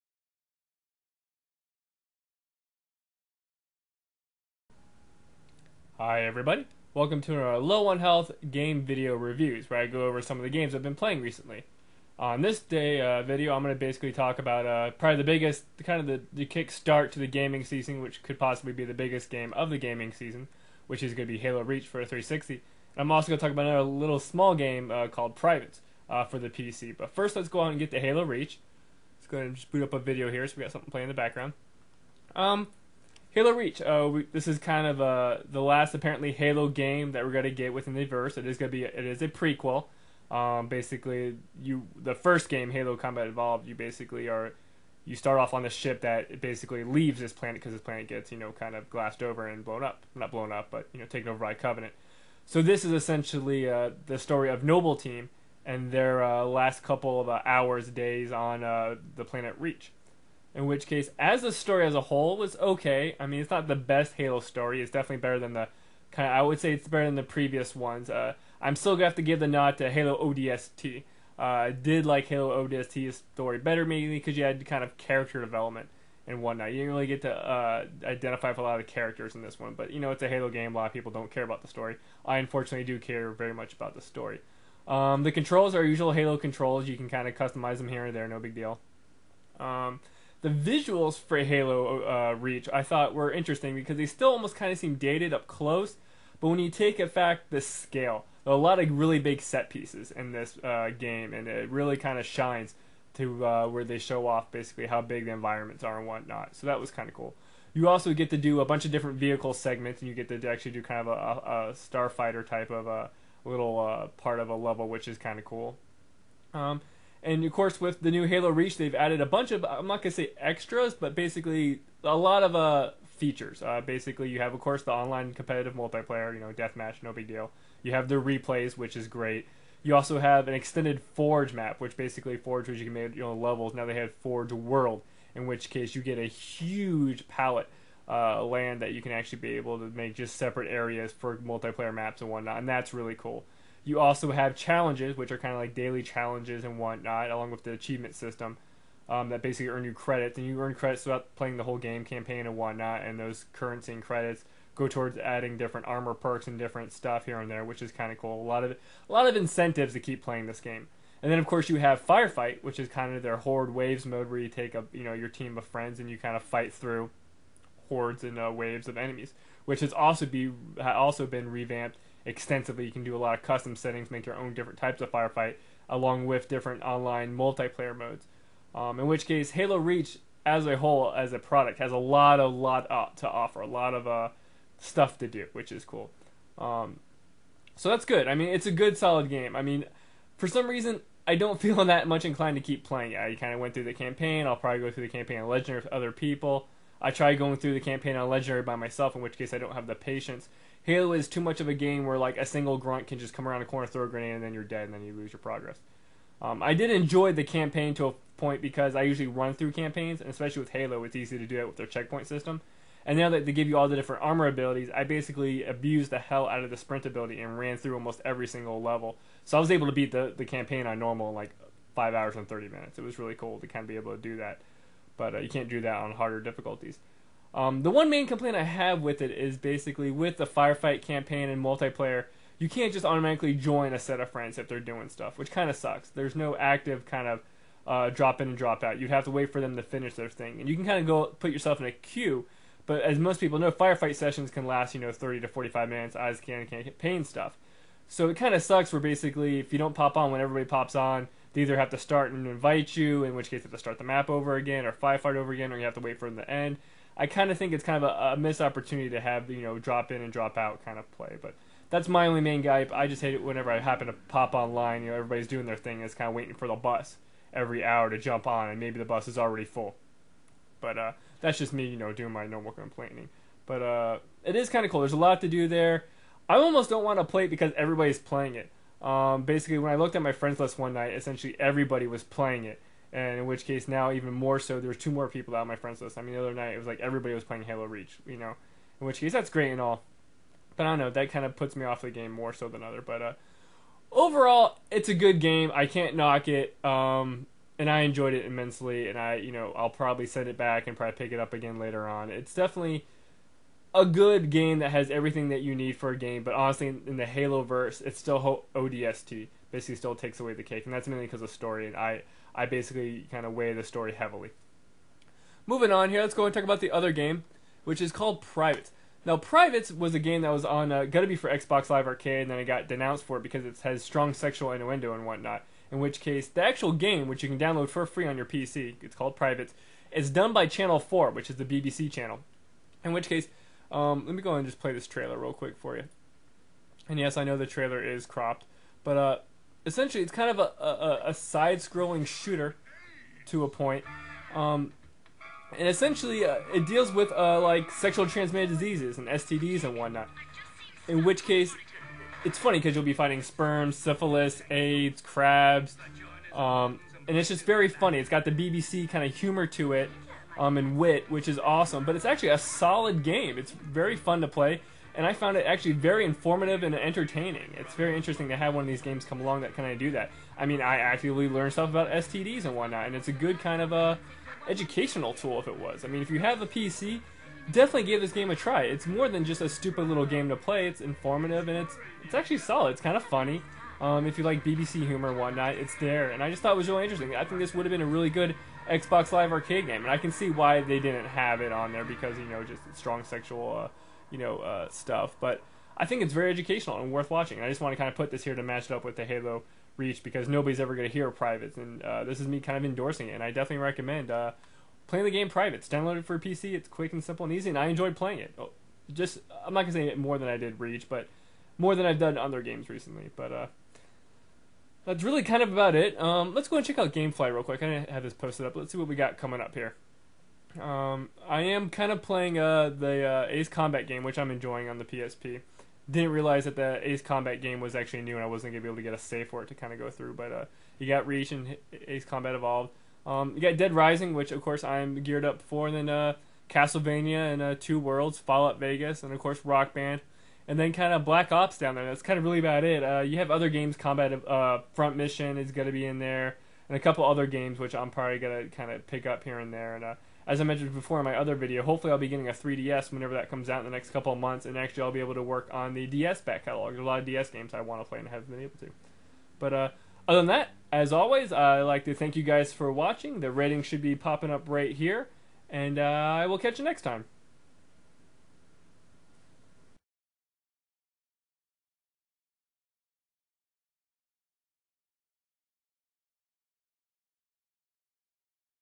LOH Halo Reach And Privates Reviews
Halo Reach P8G8D9S7T8
Visual and audio are good but I think the visuals look sort of dated. Granted the game does have to really good big set pieces that show off some awesome scale. Gameplay is pretty much standard FPS. The game does have a ton of stuff to do both off and online. I personally loved the sprint ability because that meant I could just run though the game, five and half hours on normal difficulty. The story is ok, not the best or worst in the series. Forge is greatly expanded and cool. Overall don't fell the compelled to play through it of legendary or play online despite the advances in firefight and challenges.

Privates PC T7
Good visuals and audio. Funny British humor. Good controls. Informative.